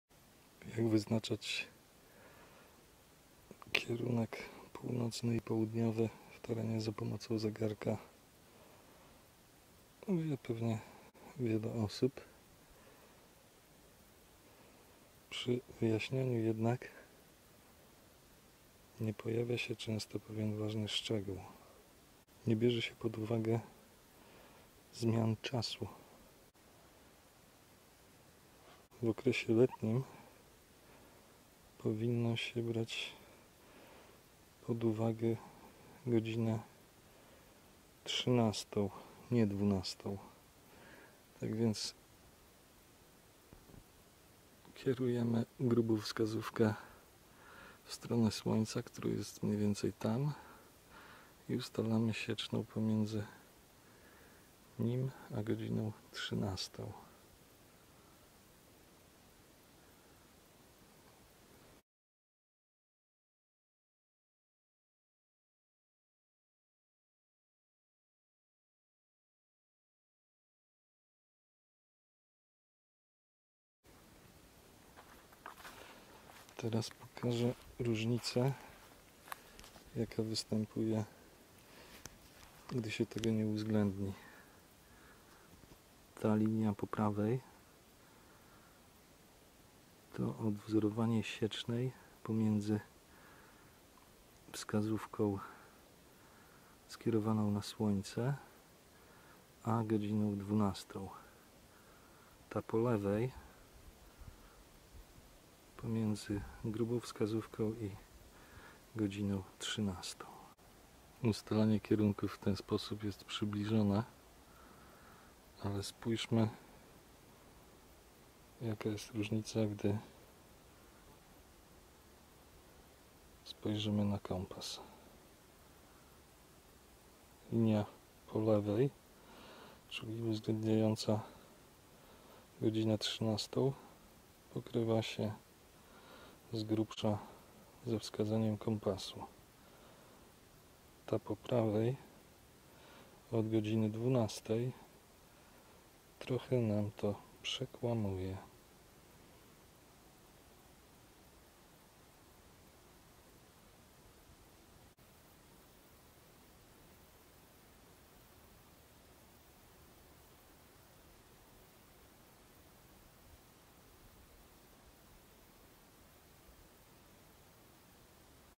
How to find North - Wristwatch as a compass - an often overlooked MISTAKE. Jak wyznaczyć północ
Often, when determining the directions of the world in this way, the change from winter to summer time is not taken into account. This is a mistake that will keep us from getting north.